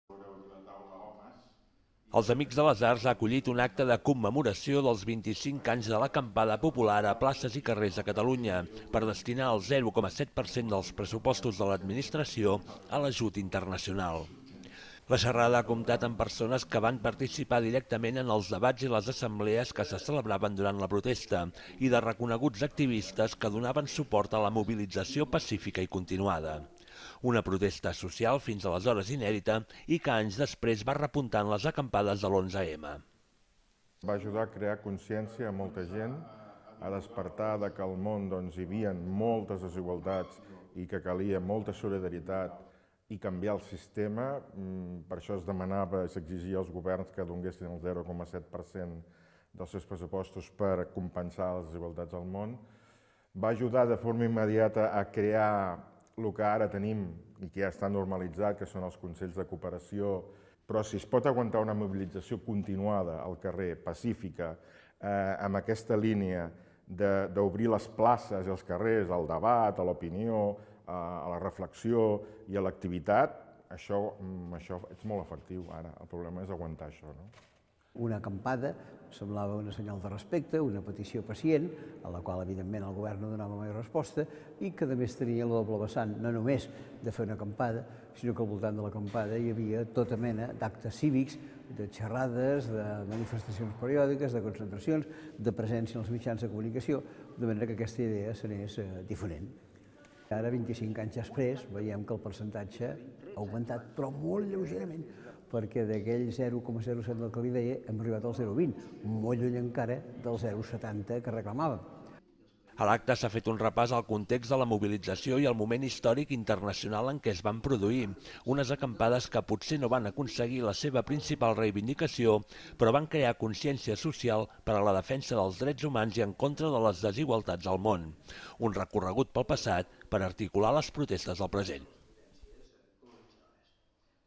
Terrassa celebra 25 anys de l'acampada del 0,7 per cent
Els Amics de les Arts ha acollit un acte de commemoració dels 25 anys de l’acampada popular a places i carrers de Catalunya, per destinar el 0,7 per cent dels pressupostos de l’administració a l’ajut internacional.